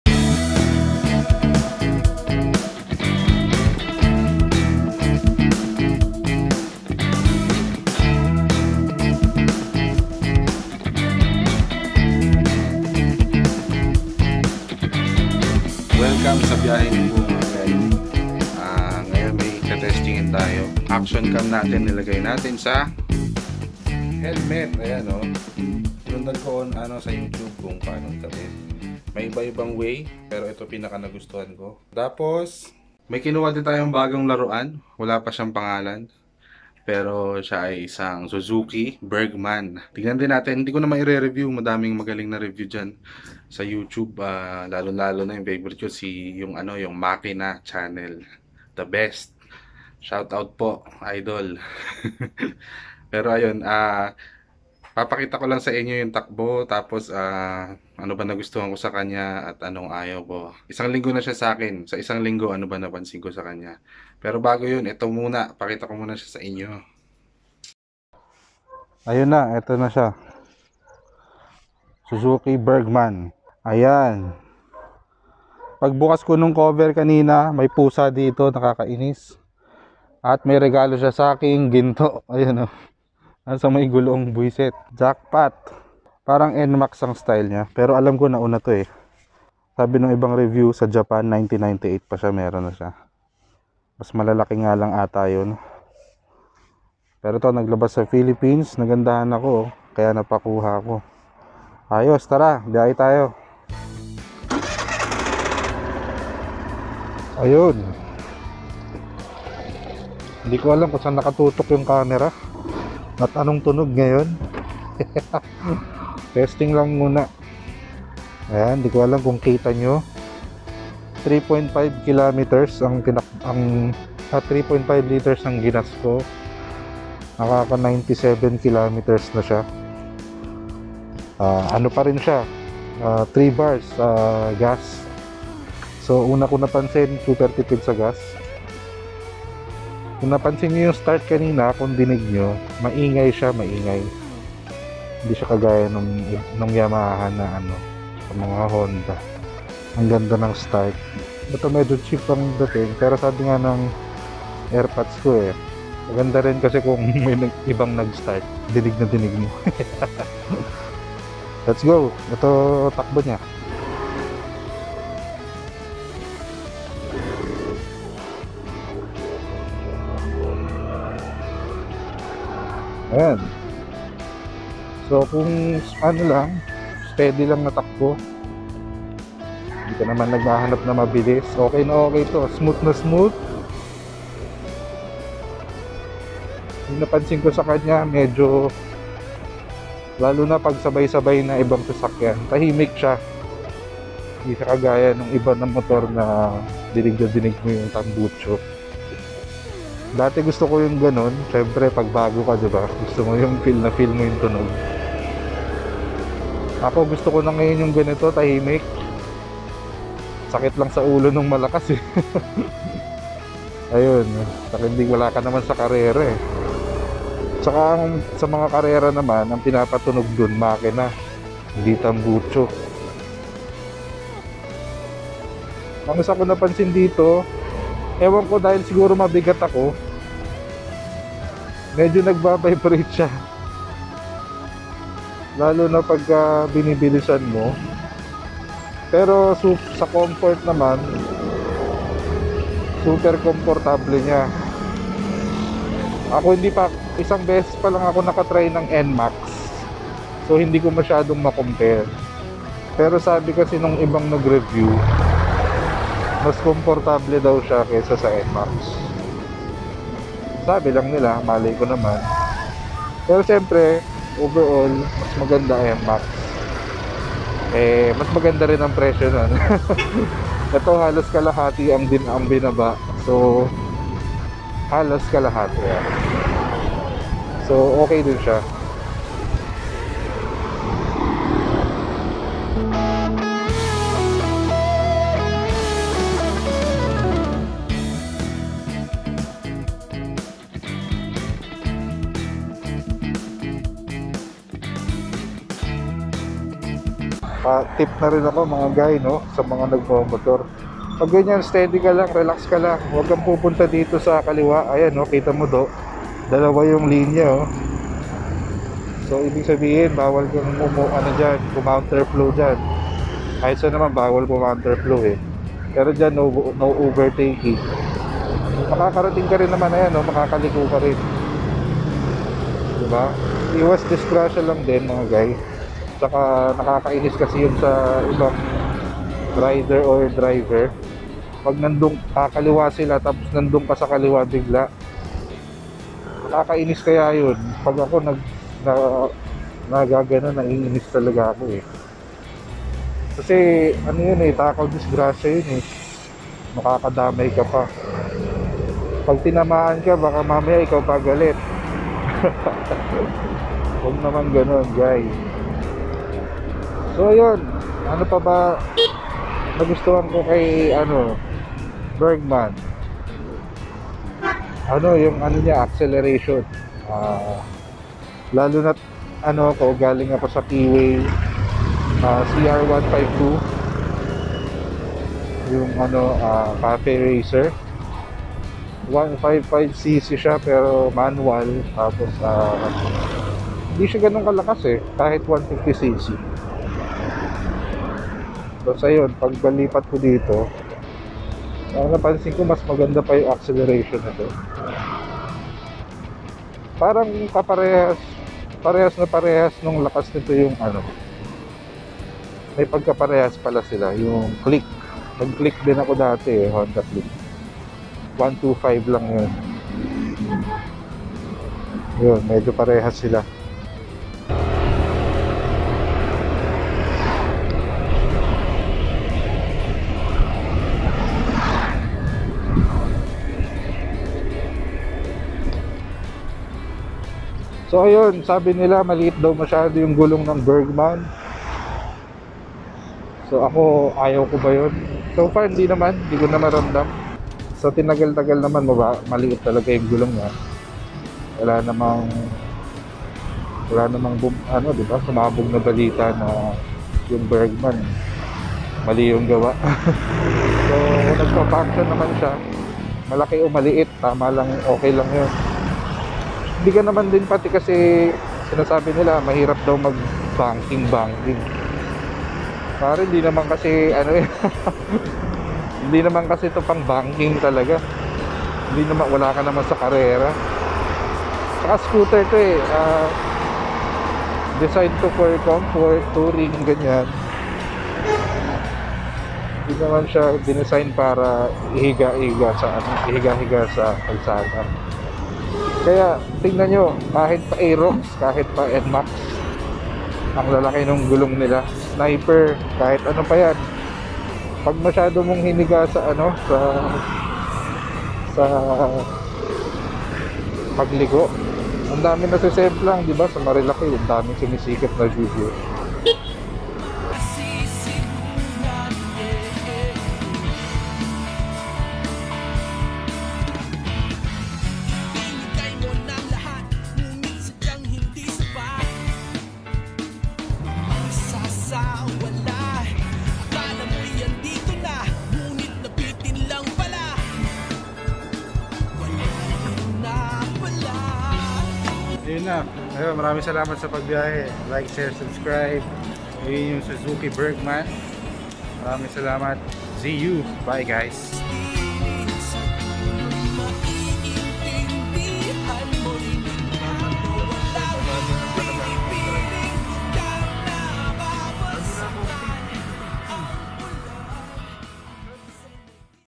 1 week with the Suzuki Burgman Street
Heyheyhey heyheyhey! Kamusta mga guy? New toy tayo ngayon at kinailangan sa mga gawain at sa taong ito. 1 week na si Burgman sakin, pakita ko lang sa inyo kung ano mga napansin ko sa kanya. Let's go!